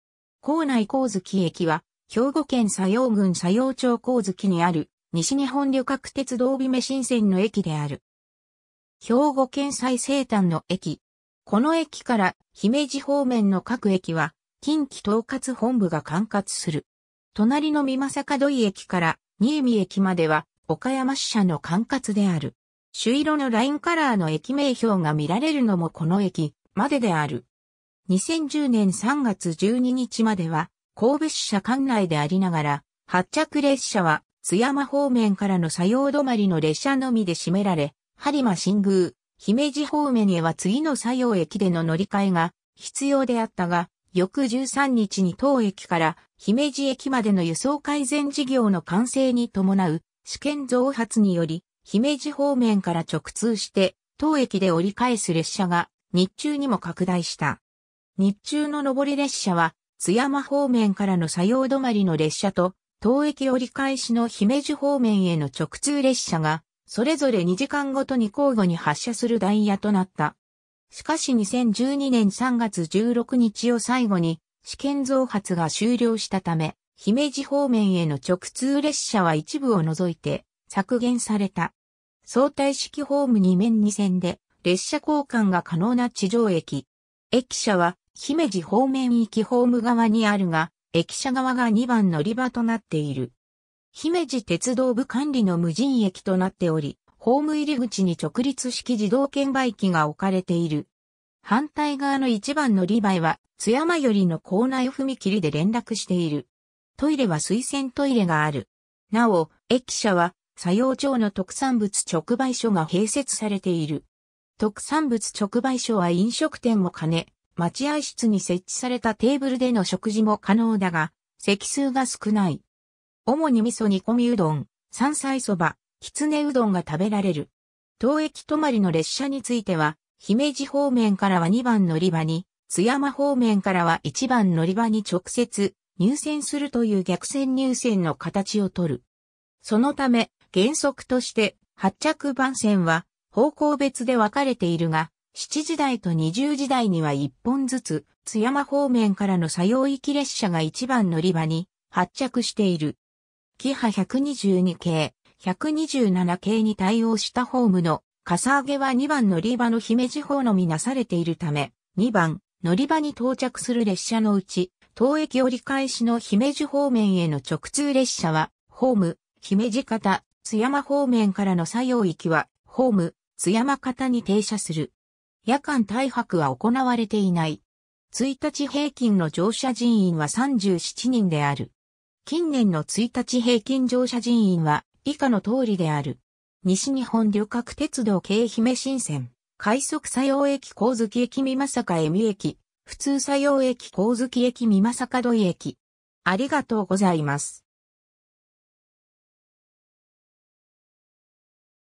上月駅
構内（2008年6月26日） 上月駅（こうづきえき）は、兵庫県佐用郡佐用町上月にある、西日本旅客鉄道（JR西日本）姫新線の駅である。
兵庫県最西端の駅。
 この駅から姫路方面の各駅は近畿統括本部（播磨高岡駅まで神戸支社姫路鉄道部）が管轄する。
隣の美作土居駅から新見駅までは岡山支社の管轄である。
朱色のラインカラーの駅名標が見られるのもこの駅までである。
 2010年（平成22年）3月12日までは神戸支社管内でありながら、発着列車は津山方面からの佐用止まりの列車のみで占められ、播磨新宮・姫路方面へは次の佐用駅での乗換が必要であったが、翌13日に当駅から姫路駅までの輸送改善（高速化）事業の完成に伴う試験増発により、姫路方面から直通して当駅で折り返す列車が日中にも拡大した。
日中の上り列車は津山方面からの佐用止まりの列車（佐用発の姫路方面への列車に接続）と、当駅折り返しの姫路方面への直通列車が、それぞれ2時間ごとに交互（合わせて1時間あたり1本）に発車するダイヤとなった。
しかし2012年3月16日を最後に試験増発が終了したため、姫路方面への直通列車は一部を除いて削減された。
 相対式ホーム2面2線で列車交換が可能な地上駅。
駅舎は姫路方面行きホーム側にあるが、駅舎側が2番のりばとなっている。
姫路鉄道部管理の無人駅となっており、ホーム入口に直立式自動券売機が置かれている。
反対側の1番のりばへは津山寄りの構内踏切で連絡している。
トイレは水洗トイレがある。
 なお、駅舎は佐用町の特産物直売所が併設されている。
特産物直売所は飲食店も兼ね、待合室に設置されたテーブルでの食事も可能だが、席数が少ない。
主に味噌煮込みうどん・山菜蕎麦・きつねうどんが食べられる。
 当駅止まりの列車については、姫路方面からは2番のりばに、津山方面からは1番のりばに直接入線するという逆線入線の形を取る。
そのため、原則として発着番線は方向別で分かれているが、7時台と20時台には1本ずつ、津山方面からの佐用行き列車が1番のりばに発着している（後者は2番のりばが姫路方面からの列車の折り返しで塞がるため）。
 キハ122系・127系に対応したホームのかさ上げは2番のりばの姫路方のみなされているため、2番のりばに到着する列車のうち、当駅折り返しの姫路方面への直通列車はホーム姫路方、津山方面からの佐用行きはホーム津山方に停車する。
 夜間滞泊は行われていない。
 1日平均の乗車人員は37人である（2019年度）。
 西日本旅客鉄道 K 姫新線 ■快速（上りは当駅止まりのみ） 佐用駅 → 上月駅 - 美作江見駅 ■普通 佐用駅 - 上月駅 - 美作土居駅